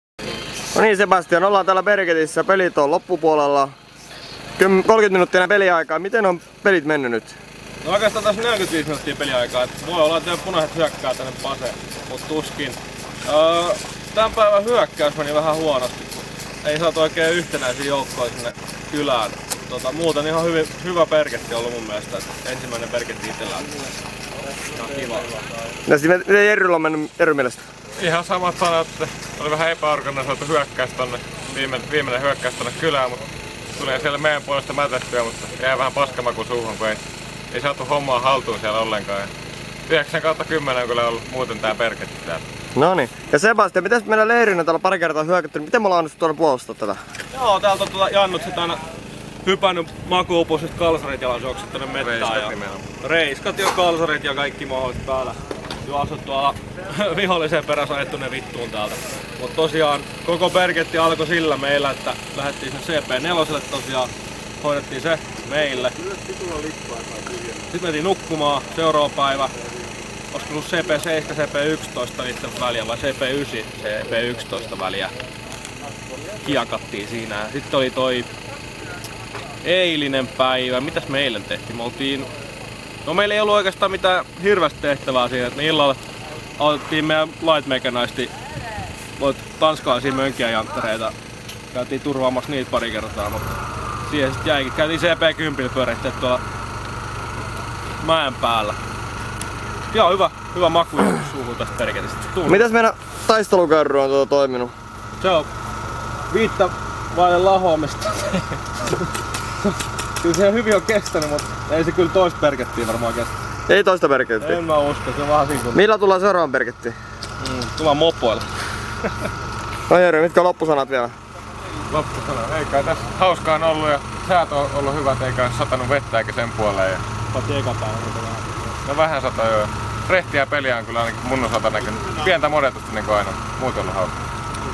Biggest airsoft event in Sweden: BERGET 13 - The Siege of Krasnovo - 15 Final words
Northern Europe's biggest airsoft event of that year, Berget 13 The Siege of Krasnovo, was organized on June 23.-27, 2015 in Viksmon, Sollefteå, Sweden.

We participated in the game on the side of GCT. Our game vehicle was an old reliable Polaris Magnum 325 4x4 ATV and a light trailer, on which we had built wooden sides.

This is our final words about the event before the aftergame.

Original video filmed on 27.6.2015